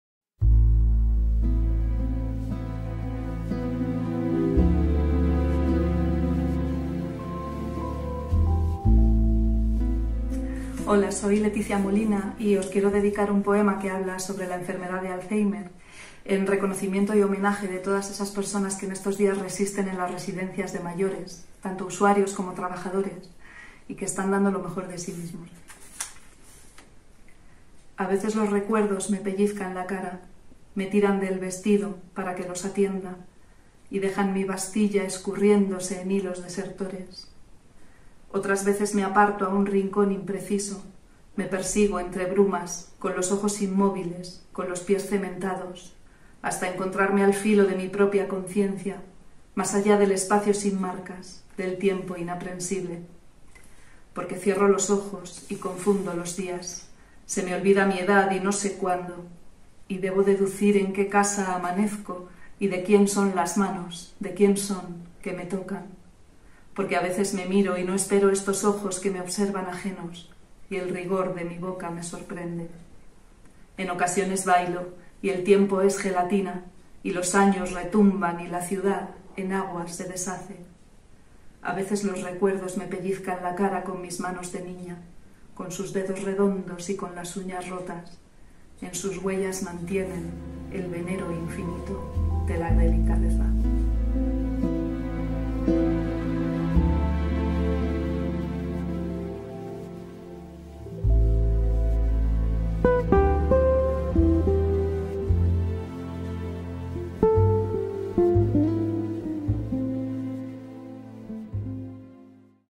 Intraversos: Leticia Medina
A iniciativa del poeta y doctor en Historia del Arte, Jesús del Real, un grupo de poetas nos regalan sus poesías para curar las heridas del alma y hacer más llevadera la cuarentena que nos permitirá vencer, entre todos, el coronavirus. Gracias por hacernos partícipes de la iniciativa Intraversos.

Porque, aunque físicamente estemos separados, "juntos somos más fuertes".